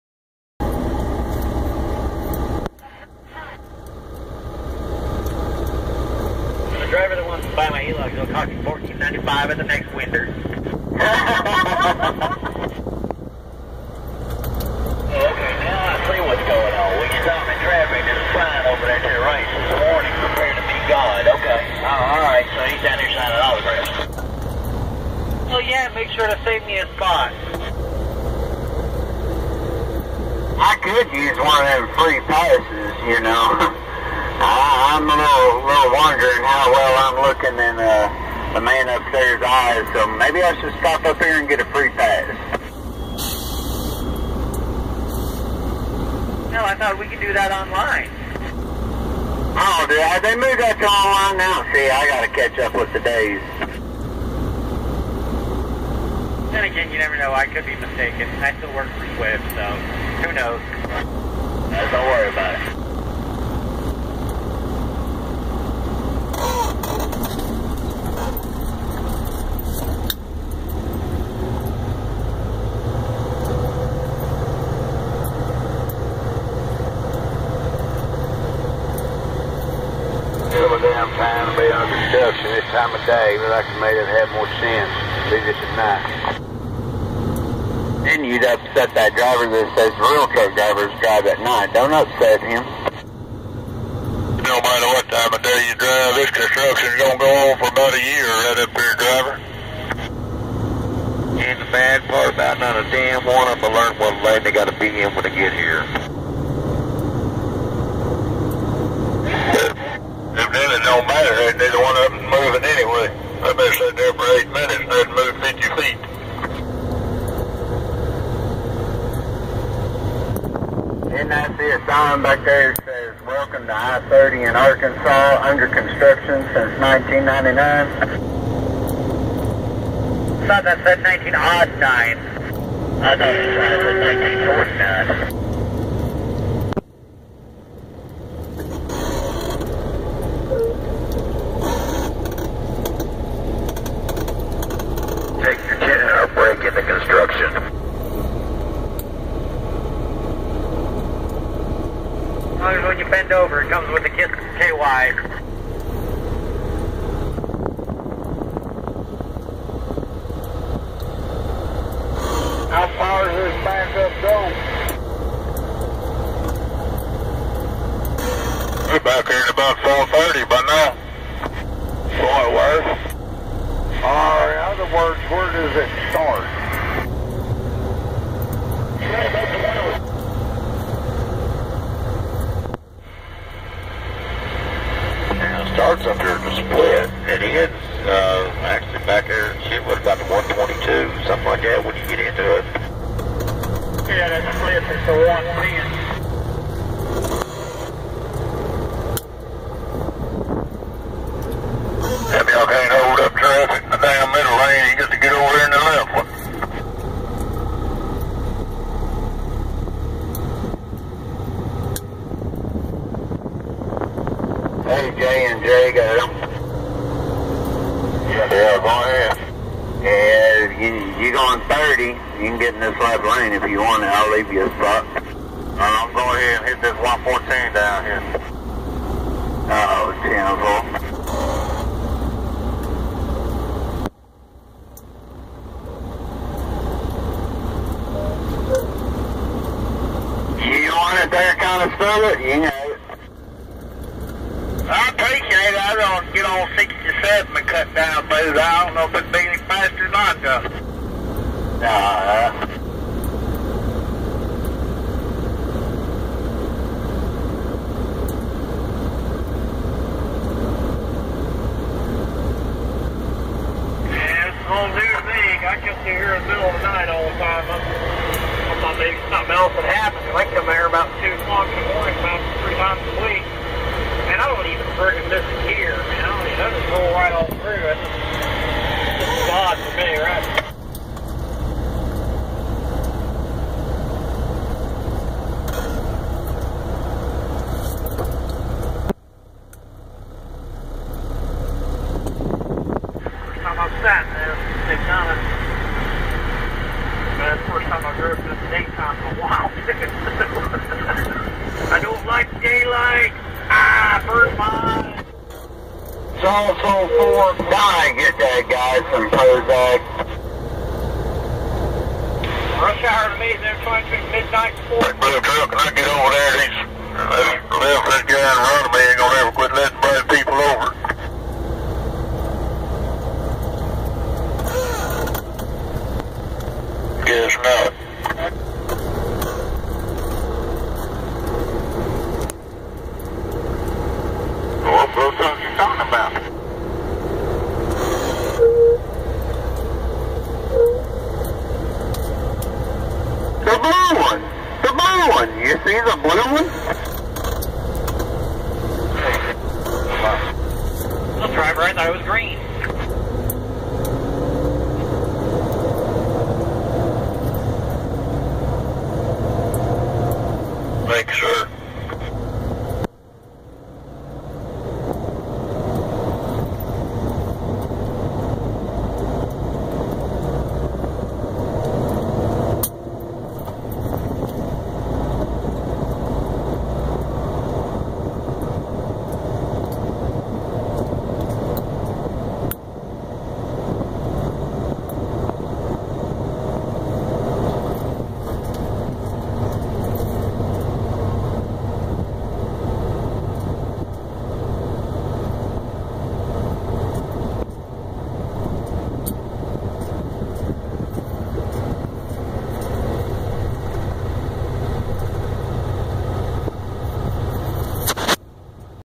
CB Radio Chatter
When truckers get stuck in traffic ,they get really bored and pissed off, and start talking shit over the radio